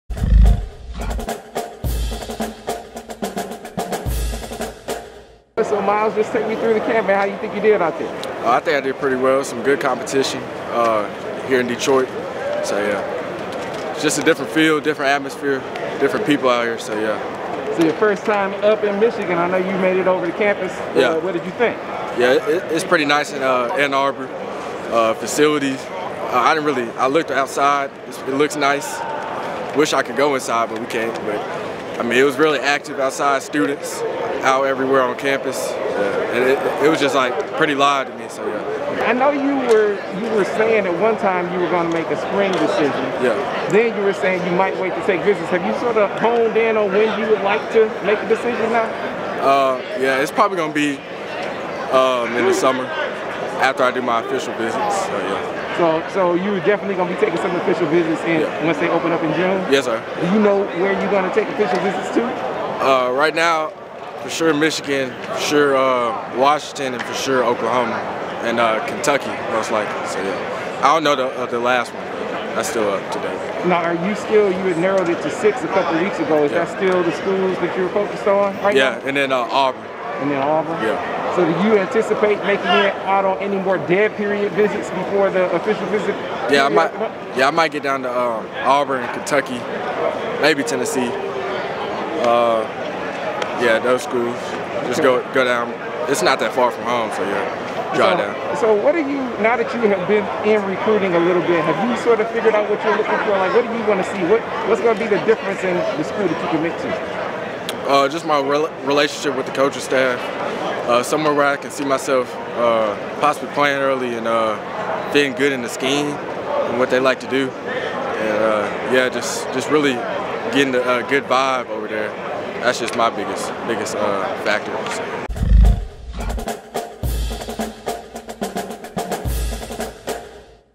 Myles Pollard talks Michigan visit; official visit plans
Brentwood (Tenn.) Ravenwood CB Myles Pollard reflects upon his dead period trip to Ann Arbor and reveals the schools he plans to take official visits to.